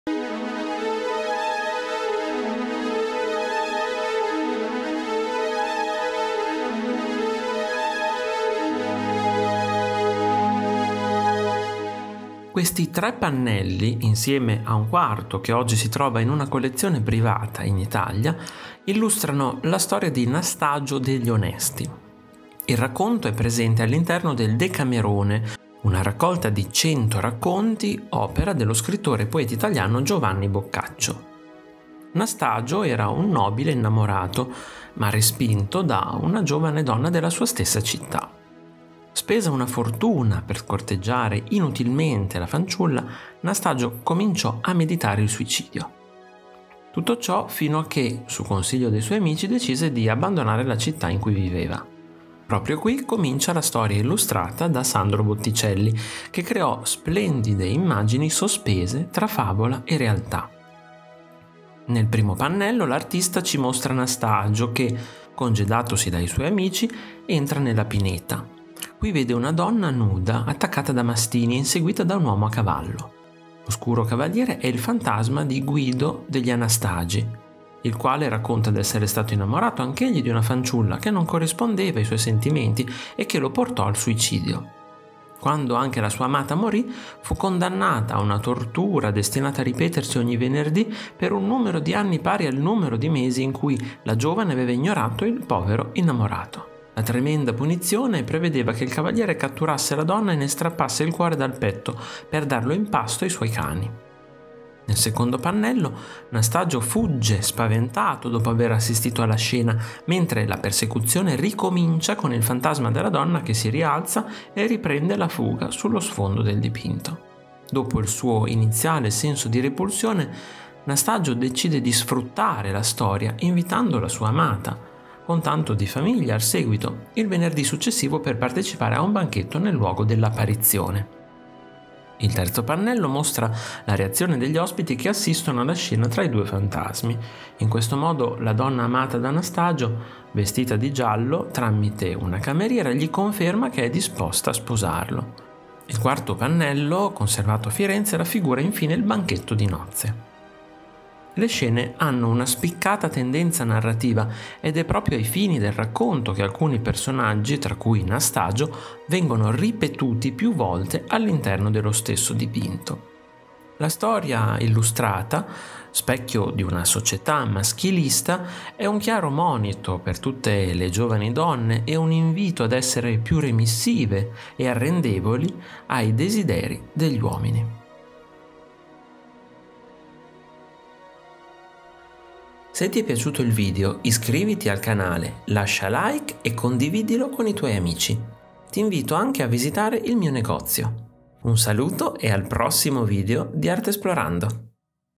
Sandro Botticelli | Nastagio degli Onesti, parte I, II e III
L'audioquadro è un modo per conoscere i più grandi capolavori della storia dell'arte. In maniera semplice e in pochi minuti. Qui vi parlerò di Nastagio degli Onesti di Sandro Botticelli ▼▼CONTINUA SOTTO▼▼

Questi tre pannelli insieme a un quarto che oggi si trova in una collezione privata in Italia, illustrano la “Storia di Nastagio degli Onesti”. Il racconto è presente all’interno del Decamerone, una raccolta di 100 racconti opera dello scrittore e poeta italiano Giovanni Boccaccio. Nastagio era un nobile innamorato, ma respinto, da una giovane donna della sua stessa città. Spesa una fortuna per corteggiare inutilmente la fanciulla, Nastagio cominciò a meditare il suicidio. Tutto ciò fino a che, su consiglio dei suoi amici, decise di abbandonare la città in cui viveva. Proprio qui comincia la storia illustrata da Sandro Botticelli che creò splendide immagini sospese tra favola e realtà.
Nel primo pannello l’artista ci mostra Nastagio che, congedatosi dai suoi amici, entra nella pineta. Qui vede una donna nuda attaccata da mastini e inseguita da un uomo a cavallo. L’oscuro cavaliere è il fantasma di Guido degli Anastagi, il quale racconta d’essere stato innamorato anch’egli di una fanciulla che non corrispondeva i suoi sentimenti e che lo portò al suicidio. Quando anche la sua amata morì, fu condannata a una tortura destinata a ripetersi ogni venerdì per un numero di anni pari al numero di mesi in cui la giovane aveva ignorato il povero innamorato. La tremenda punizione prevedeva che il cavaliere catturasse la donna e ne strappasse il cuore dal petto per darlo in pasto ai suoi cani.

LETTURE CONSIGLIATE
➡ Botticelli, Giunti Editore
➡ Botticelli, Taschen
➡ Botticelli e il suo tempo